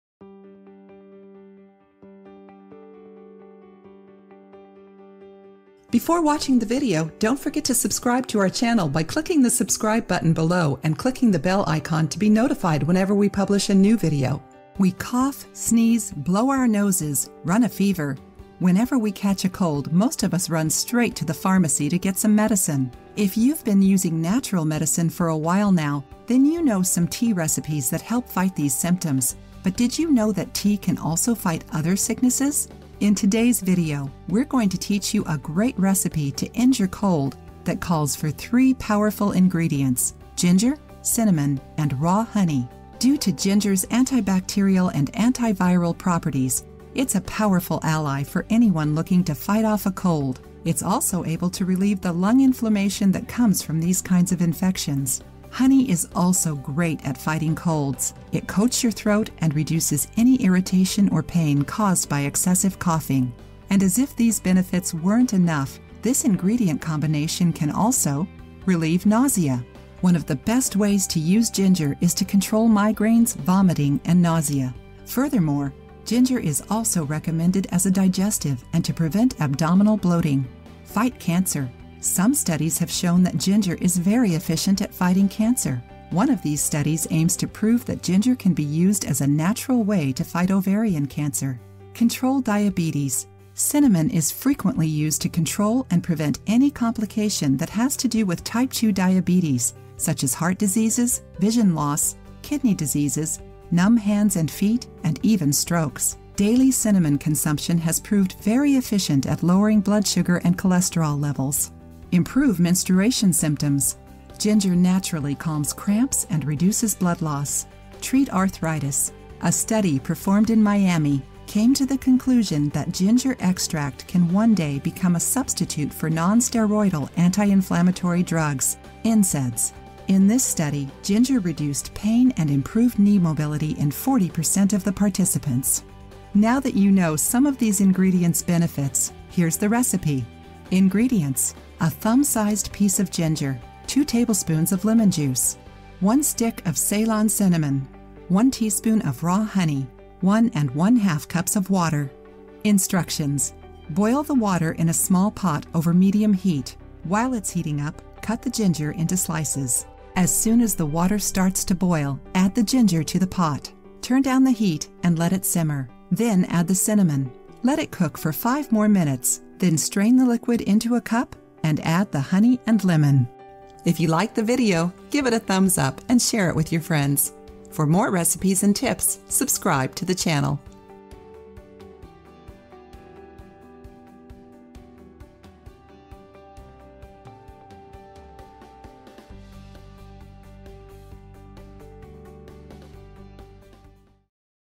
This Ginger, Honey and Cinnamon Tea Fights Colds, Sore Throats and More!
This tea recipe helps to keep weight off and strengthen your immune system with ingredients like cinnamon, ginger, and honey.

We cough, sneeze, blow our noses, run a fever... Whenever we catch a cold most of us run straight to the pharmacy to get some medicine. If you've been using natural medicine for a while now, then you know some tea recipes that help fight these symptoms. But did you know that tea can also fight other sicknesses?

In today's video, we're going to teach you a great recipe to end your cold, that calls for 3 powerful ingredients: ginger, cinnamon, and raw honey.

Due to ginger's antibacterial and anti-viral properties, it's a powerful ally for anyone looking to fight off a cold. It's also able to relieve the lung inflammation that comes from these kinds of infections.

Honey is also great at fighting colds. It coats your throat and reduces any irritation or pain caused by excessive coughing.

And as if these benefits weren't enough, this ingredient combination can also:

Relieve nausea
Fight Cancer
Control Diabetes
Improve menstruation symptoms
Treat arthritis

Now that you know some of these ingredients' benefits, here's the recipe:

Ingredients

- A thumb sized piece of ginger;
- 2 tablespoons of lemon juice;
- 1 stick of Ceylon cinnamon;
- 1 teaspoon of raw honey;
- 1 and ½ cups of water.

Instructions

Boil the water in a small pot, over medium heat. While it's heating up, cut the ginger into slices. As soon as the water starts to boil, add the ginger to the pot. Turn down the heat, and let it simmer. Then, add the cinnamon. Let it cook for 5 more minutes, then strain the liquid into a cup and add the honey and lemon.